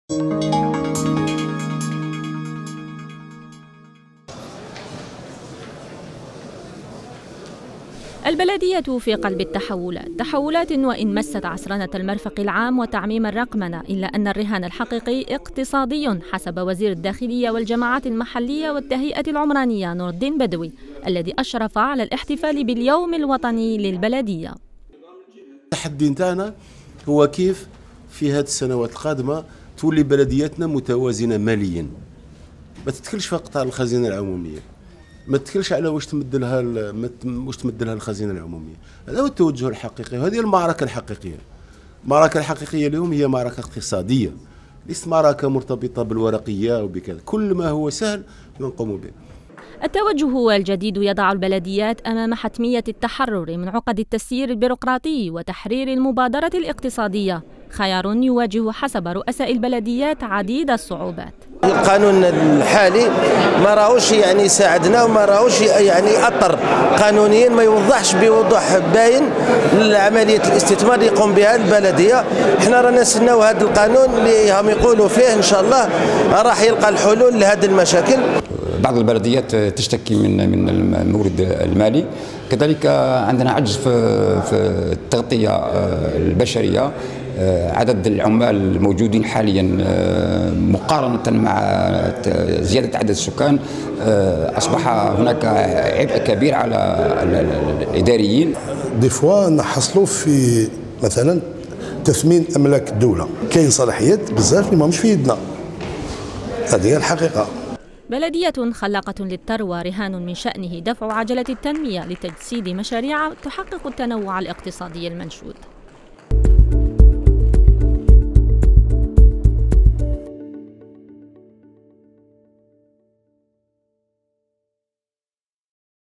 البلديات أمام حتمية تحرير المبادرة الاقتصادية
الجزائر( وأج) - أحيت الجزائر اليوم الوطني للبلديات من خلال الوقوف عند التوجه الجديد الرامي لجعل البلدية خلاقة للثروة من خلال تحرير المبادرة الاقتصادية .